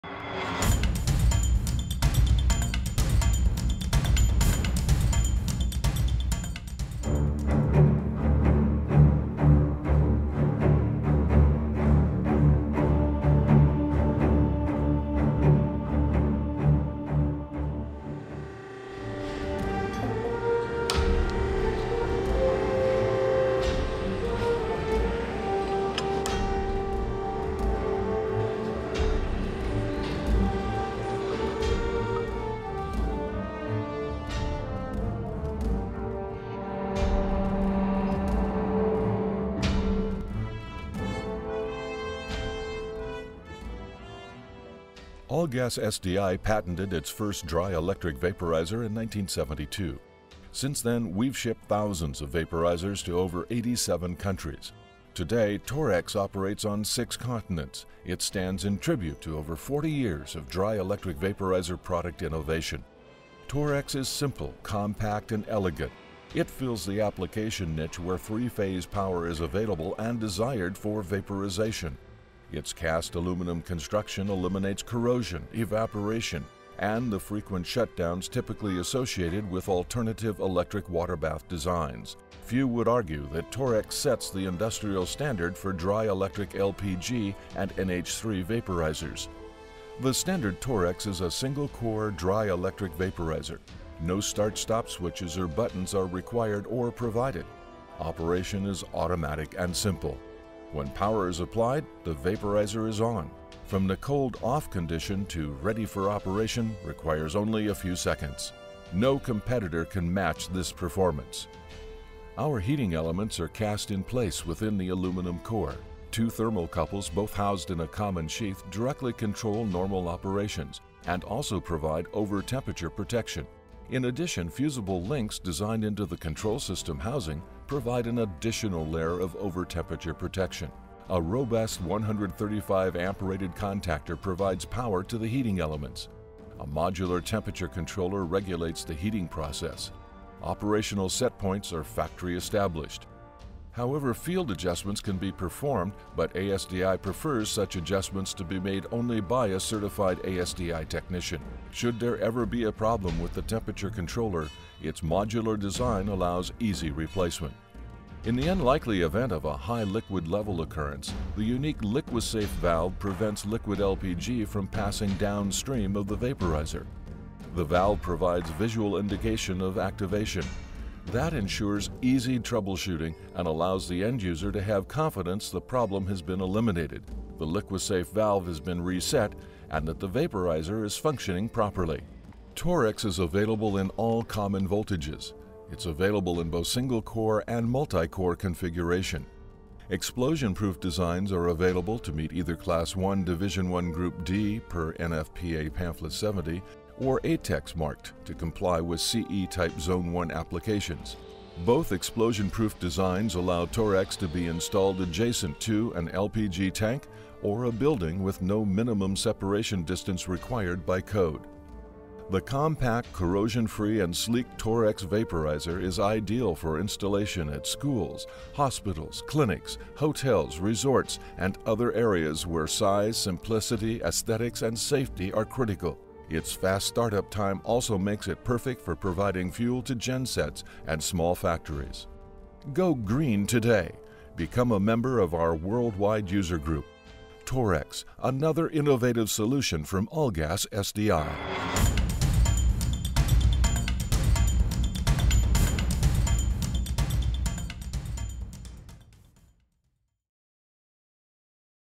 Torrexx
Algas-SDI patented its first dry electric vaporizer in 1972. Since then we've shipped thousands of vaporizers to over 87 countries. Today Torrexx operates on 6 continents. it stands in tribute to over 40 years of dry electric vaporizer product innovation.
 Torrexx is simple, compact and elegant. It fills the application niche where 3-phase power is available and desired for vaporization. Its cast aluminum construction eliminates corrosion, evaporation and the frequent 'shutdowns' typically associated with alternative electric waterbath designs. Few would argue that Torrexx sets the industrial standard for dry electric LPG and NH3 vaporizers.
The standard Torrexx is a single core, dry, electric vaporizer.  No start/stop switches or buttons are required or provided. Operation is automatic and simple. When power is applied, the vaporizer is On. From the cold OFF condition to "ready for operation" requires only a few seconds!  No competitor can match this performance.
Our heating elements are cast in place within the aluminum core. Two thermocouples, both housed in a common sheath,  directly control the normal operation and  also provide over-temperature protection.  In addition, fusible links designed into the Control System Housing provide an additional layer of over-temperature protection.
A robust 135amp rated contactor provides power to the heating elements. A modular temperature controller regulates the heating process. Operational set points are factory established. However, field adjustments can be performed but ASDI prefers such adjustments be made only by a certified ASDI Technician. Should there ever be a problem with the temperature controller, its modular design allows easy replacement.
In the unlikely event of a "high liquid level occurrence,"  the unique Liqui-Safe Valve prevents liquid LPG from passing downstream of the vaporizer.  The valve provides visual indication of activation. This ensures easy troubleshooting and allows the end-user to have confidence the problem has been eliminated, the Liqui-Safe Valve has been reset and that the vaporizer is functioning properly.

Torrexx is available in all common voltages.  It is available in both single core and multi-core configuration. Explosion proof designs are available to meet either Class I, Division I, Group D (per NFPA Pamphlet 70) or, ATEX marked to comply with CE type Zone I applications.  Both explosion proof designs allow Torrexx to be installed adjacent to an LPG tank or a building with no minimum separation distance required by Code.
Compact, corrosion-free and sleek Torrexx Vaporizer is an ideal installation at schools, hospitals, clinics, hotels, resorts and other areas where size, simplicity, aesthetics and SAFETY are critical. It's fast start up time also makes it perfect for providing fuel to gensets and small factories.
Go "Green" today! Become a member of our worldwide user group!   Torrexx - another innovative solution from Algas-SDI.